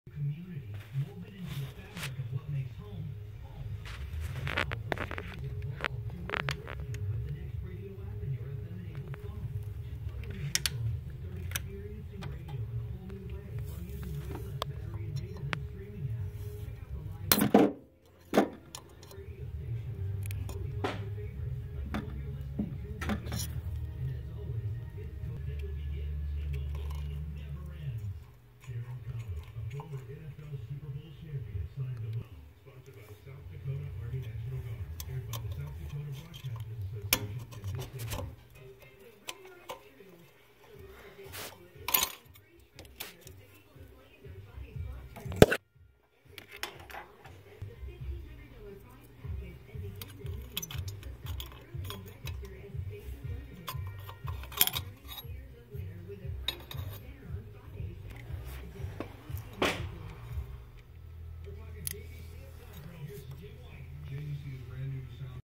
Cummins turbo silencer ring removal
How to make your HX35 turbo whistle! Short video taking the silencer ring out of a turbo found on many cummins motors.